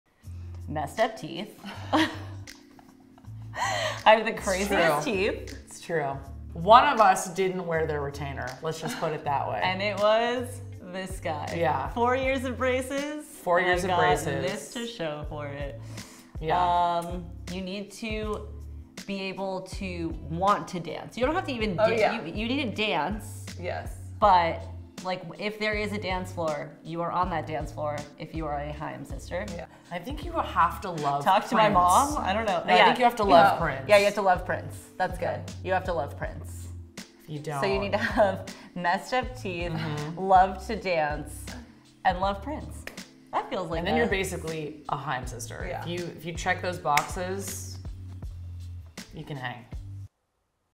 Want To Be Haim's Fourth Band Member? Watch On... | ELLE UK
Haim reveal what the requirements are to be their fourth band member.